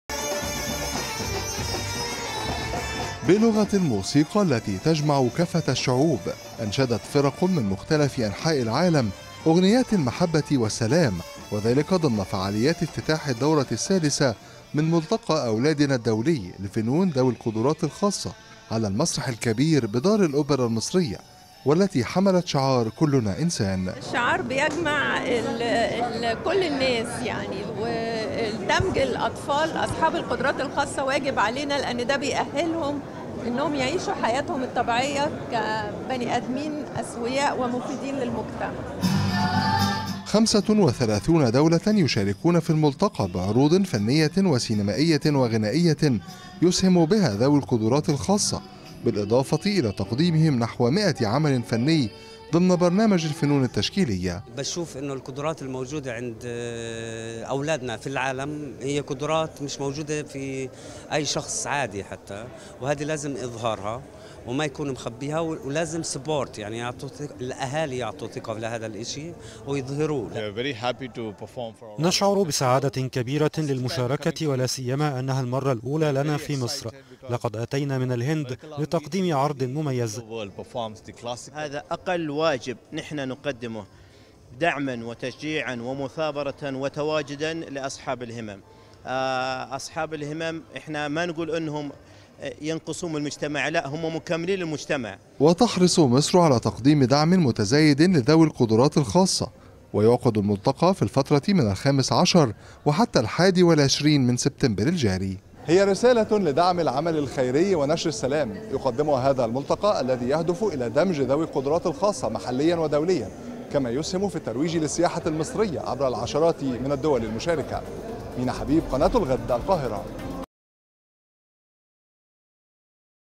"كلنا إنسان".. تفاصيل افتتاح ملتقى أولادنا الدولي في مصر لذوي القدرات الخاصة
شهد المسرح الكبير بدار الأوبرا_المصرية افتتاح الدورة السادسة لملتقى أولادنا الدولي لذوي القدرات الخاصة. وتحمل هذه الدورة عنوان "كلنا إنسان" ويشارك بها خمس وثلاثون دولة عربية وأجنبية.
المزيد من التفاصيل في سياق تقرير مراسل الغد من القاهرة، مينا حبيب.